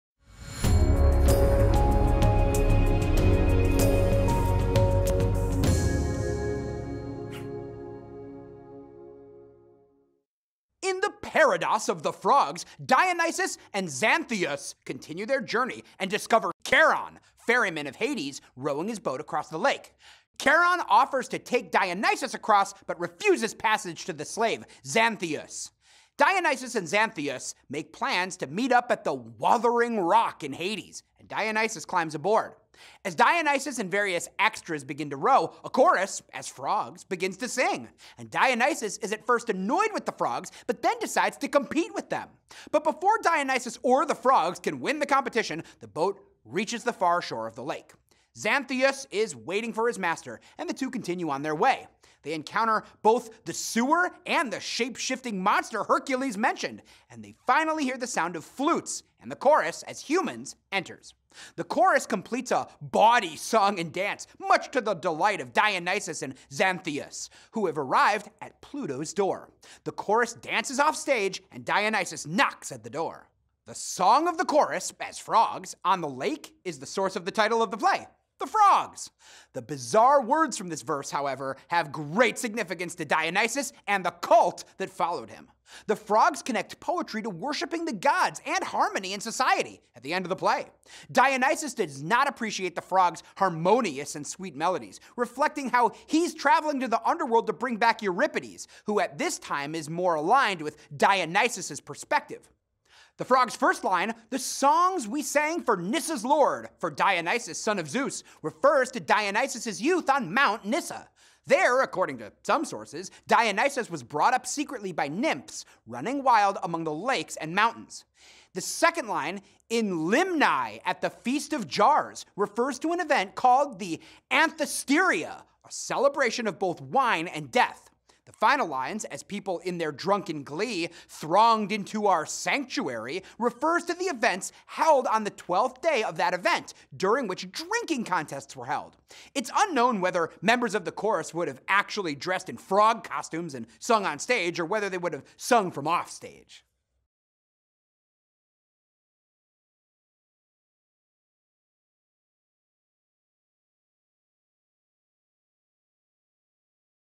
The Frogs by Aristophanes| Parodos
Summarize videos instantly with our Course Assistant plugin, and enjoy AI-generated quizzes Aristophanes's play The Frogs explained with act summaries in just a few minutes!

Course Hero Literature Instructor Russell Jaffe provides an in-depth summary and analysis of the Parodos from Aristophanes's play The Frogs.

Download the free study guide and infographic for The Frogs

In Aristophanes's satirical play The Frogs, the god Dionysus journeys to Hades dressed as the hero Hercules. He wishes to converse with playwright Euripides, who has recently died.

Accompanied by his slave Xanthias, with whom he switches identities on several occasions, he crosses the water into the underworld.

The two have comic and serious adventures before reaching Pluto’s court, including an encounter with a chorus of frogs, who comically step in for the traditional chorus of Greek theater.

There, Dionysus then serves as a judge of the poetic skills of Euripides and Aeschylus, another playwright.

Aeschylus, who represents a more conservative theatrical style, eventually prevails.  He does so by suggesting that Aristophanes is defending the tradition of Old Comedy against the innovations of the likes of Euripides.

Greek playwright Aristophanes’s play The Frogs was first performed in 405 BCE. Best-remembered of the Old Comedy playwrights of ancient Greece, most of his work was satirical and had political themes. His characters included gods, demigods, contemporary and recently deceased politicians, and playwrights. He used birds, clouds, and frogs for his imaginative choruses.

The satirical play The Frogs contain many important themes, including a return to the old ways, as Aeschylus, who argues that Athenian survival is dependent on the tradition of his works wins the battle; identity, as it is best to not pretend to be something you are not; and the importance of poetry and drama, as the role of poetry and drama was particularly significant in ancient Greece, when other forms of mass communication were in their infancy. Important symbol includes the lion skin and the scale.